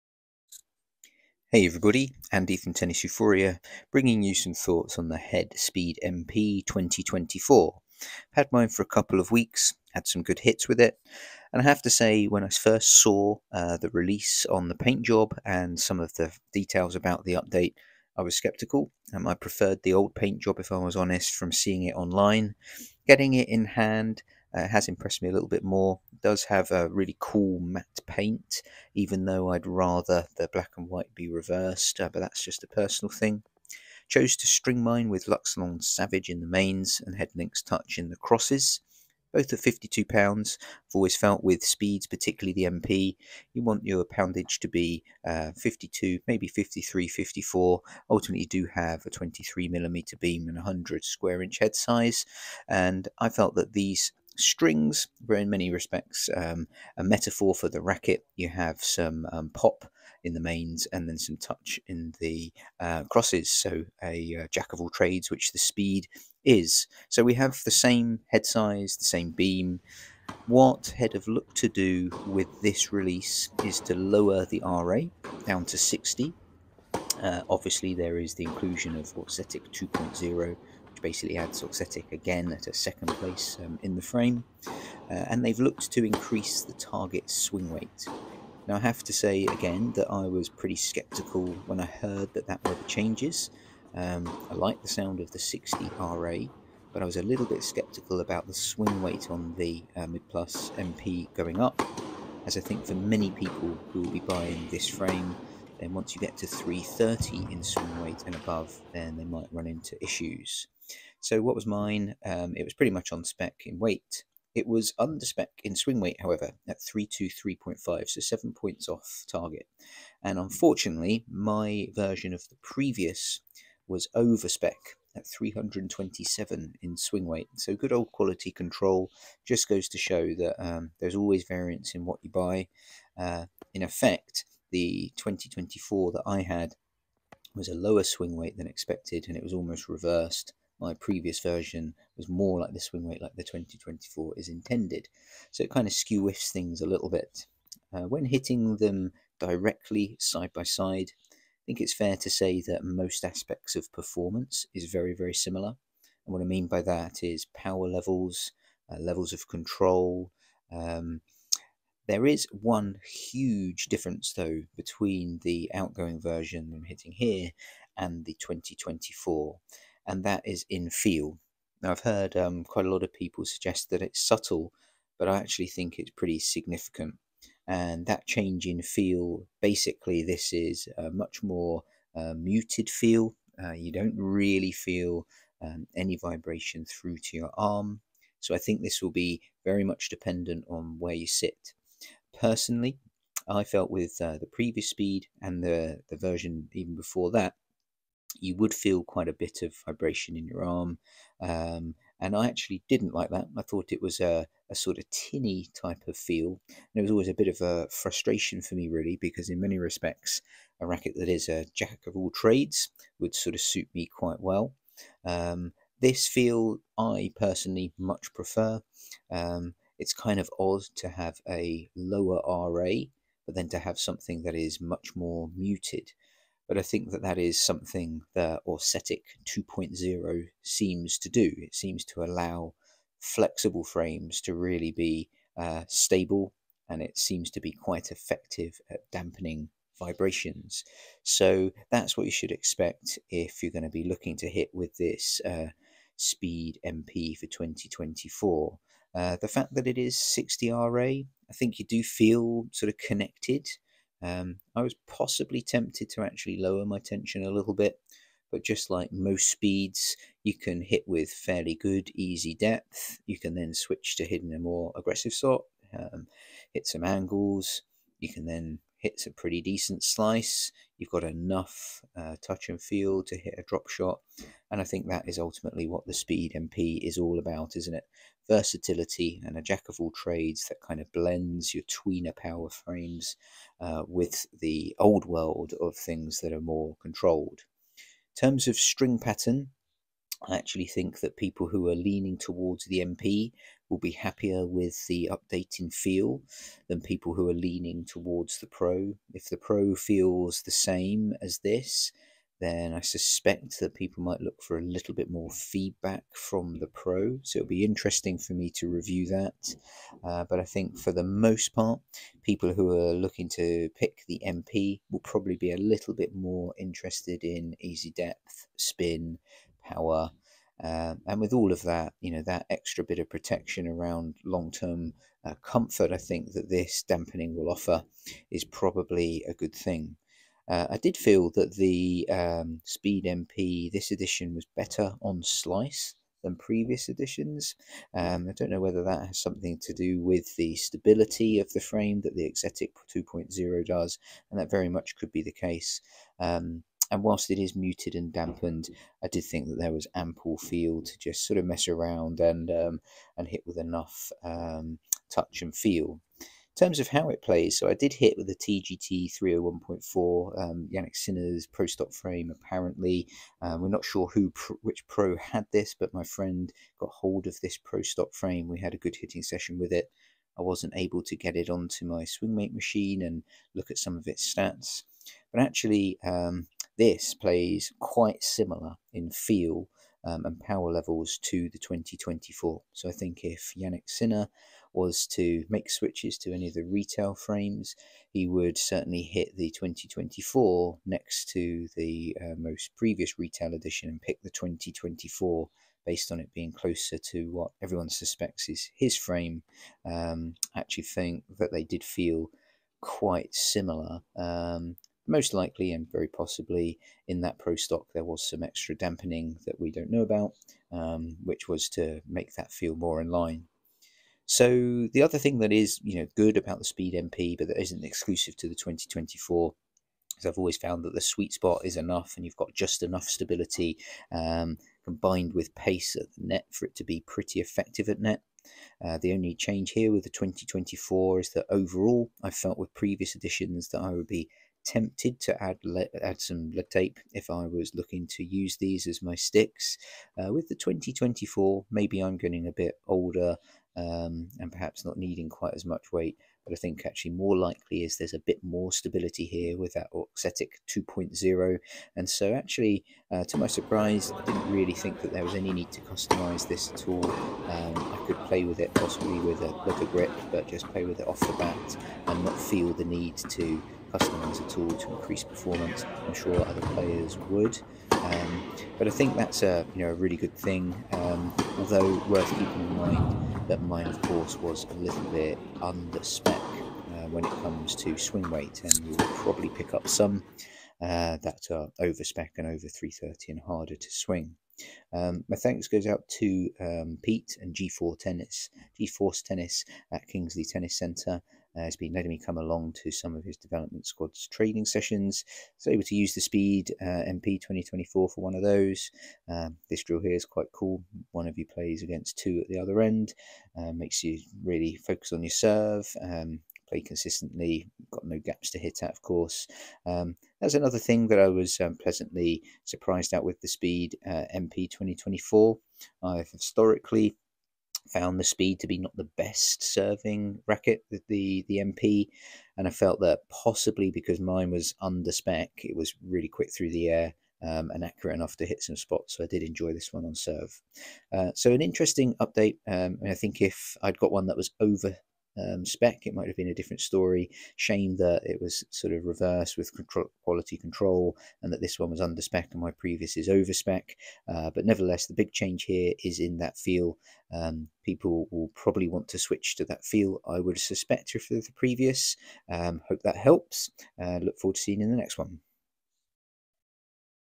Head Speed MP 2024 Tennis Racquet / Racket review
A review of the Head Speed MP 2024.
In the vide I compare the new speed to the outgoing version and consider what changes have been made.
I look at its characteristics and test the racket, against the previous at times.